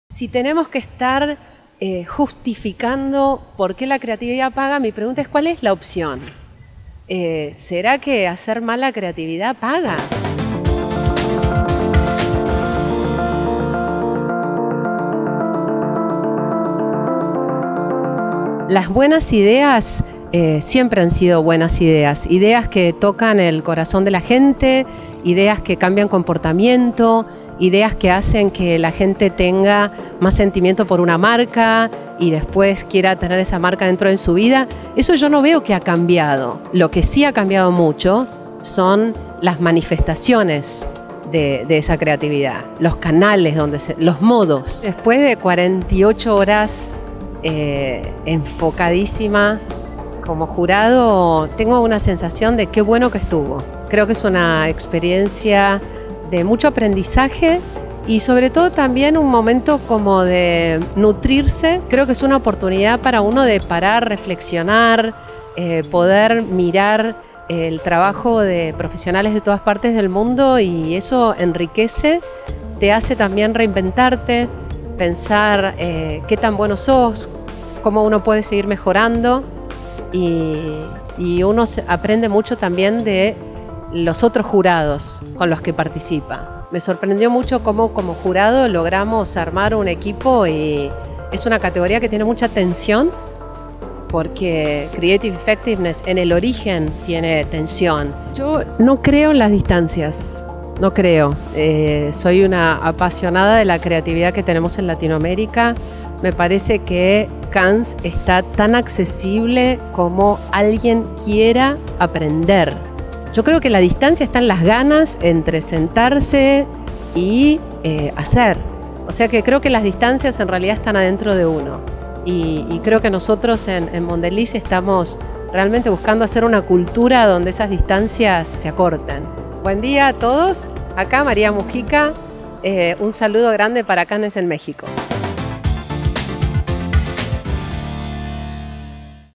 María Mujica "Soy una apasionada de la creatividad latinoamericana"
María Mujica, Dir. Regional LATAM Mktg Estratégico y Comunicación, Mondalez. Juez de la categoría Creative Effectivenness en Cannes Lions International Advertising Festival.